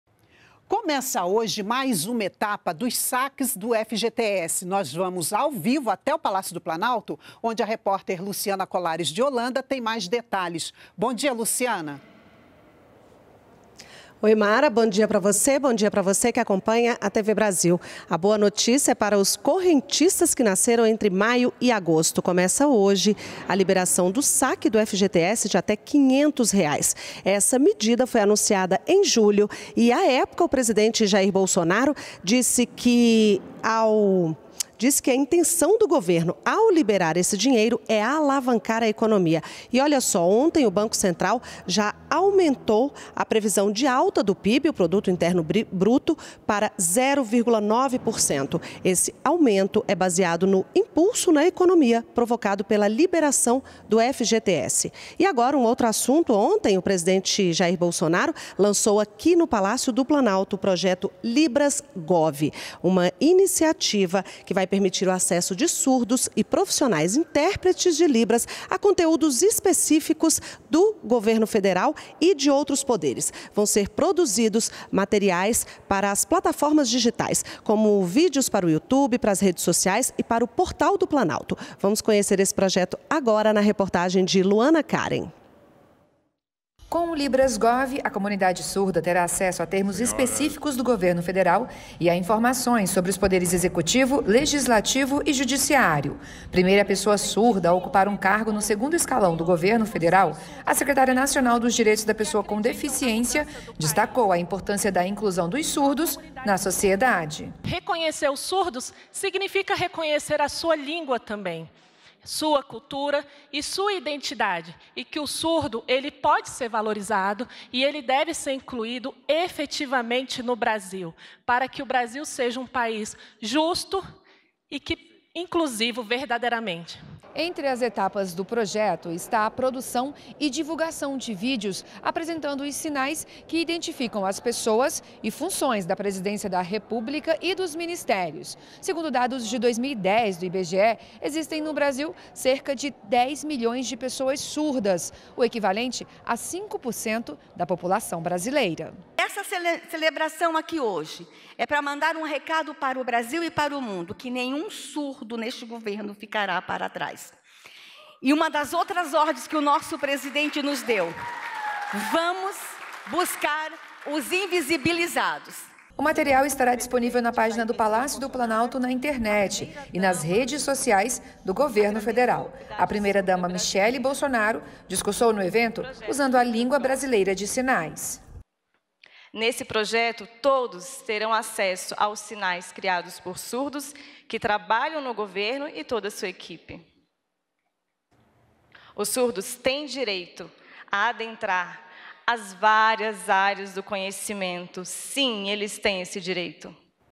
Presidente lança o projeto Libras Gov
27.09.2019 - O presidente Jair Bolsonaro lançou, em Brasília, o projeto Libras Gov. A iniciativa vai permitir o acesso de surdos e profissionais, intérpretes de Libras, a assuntos específicos do Governo Federal, e de outros poderes.